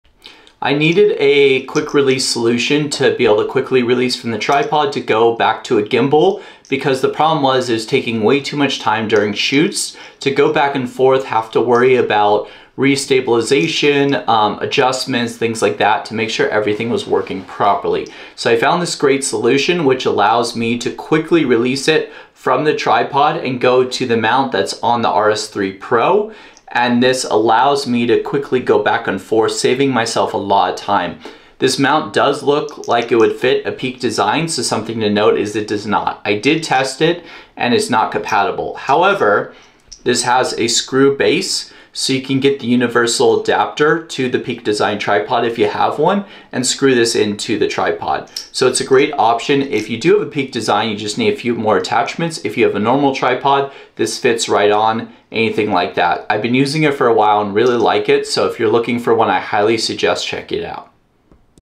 Why I bought the ULANZI F38 Camera Quick Release Plate
How I use the ulanzi f38 camera quick release plate